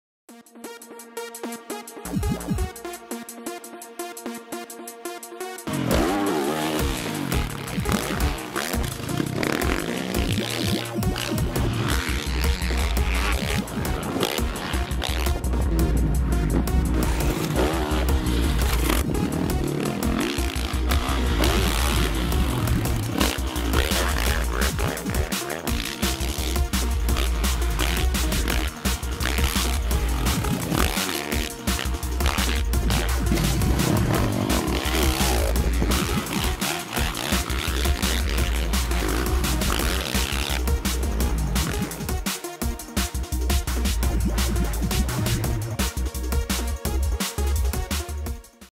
2011_JAKE MOSS Fox
Jake Moss is Welcomed to the Fox Team for Super X 2011.